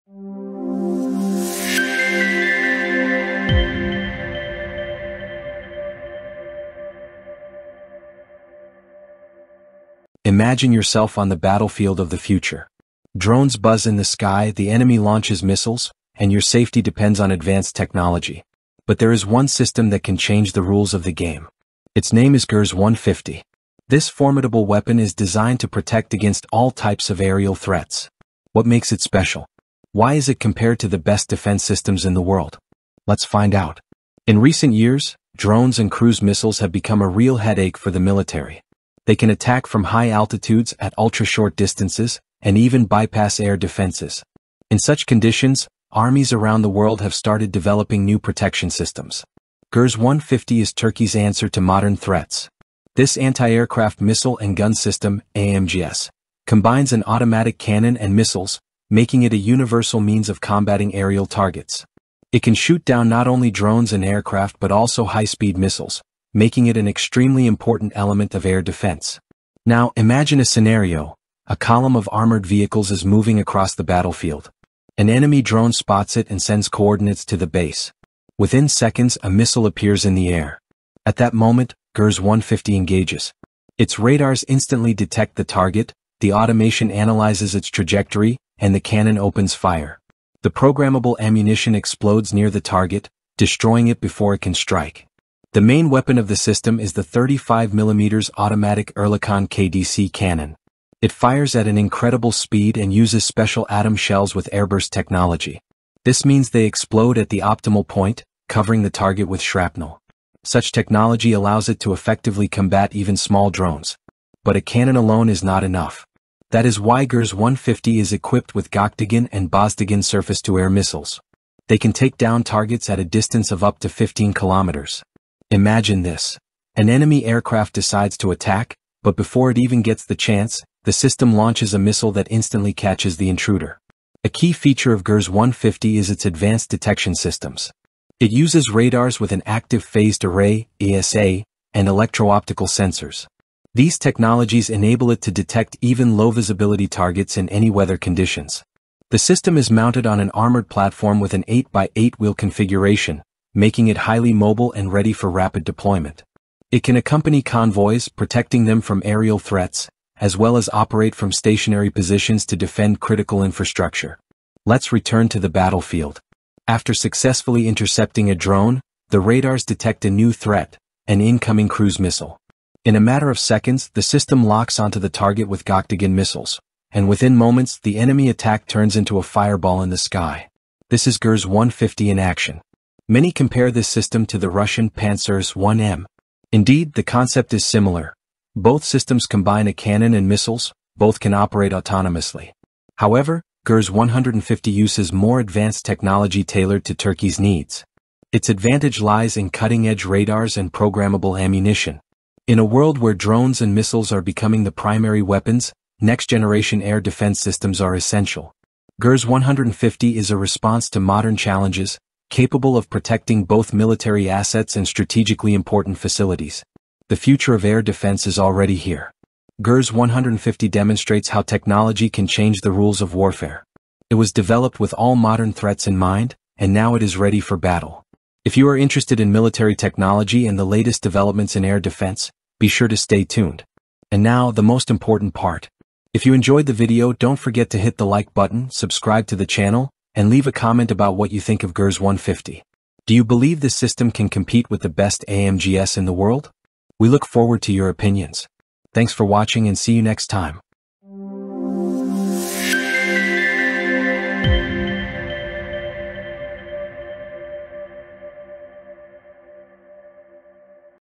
Gürz 150 The Air Defense System That Changes the Game!
The Gürz 150 air defense system is Turkey’s cutting-edge response to modern aerial threats, combining a 35mm automatic cannon with Gökdoğan and Bozdoğan surface-to-air missiles. Equipped with AESA radar and electro-optical sensors, it can detect and neutralize drones, cruise missiles, and enemy aircraft with precision. Its high mobility (8×8 platform) allows it to protect both military convoys and strategic locations. How does it compare to systems like Pantsir-S1M? Watch the video to explore its technology, capabilities, and battlefield impact!